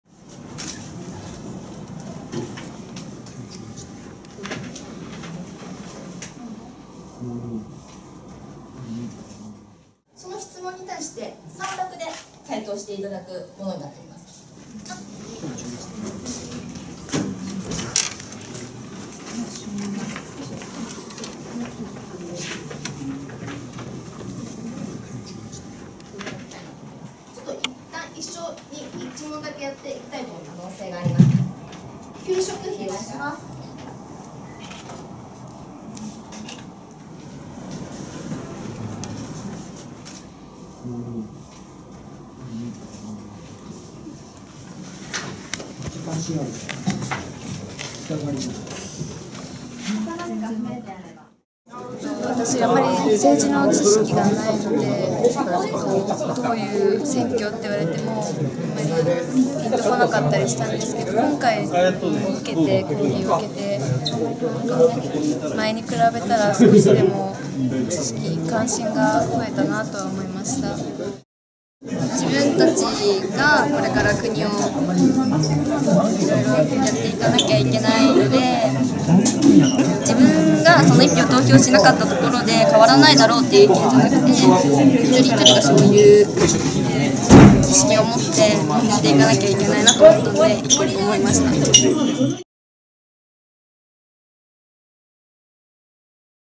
政治への関心、高３に聞く ヤフーがアンケート実施
選挙権年齢が１８歳以上に引き下げられる参院選を前に、若年層向けの選挙特集サイトを作るため、検索大手ヤフーと早稲田大マニフェスト研究所が９日、通信制の「クラーク記念国際高校」横浜キャンパス（横浜市）で、３年生３２人を対象に政治への関心や意見を聞くアンケートを実施した。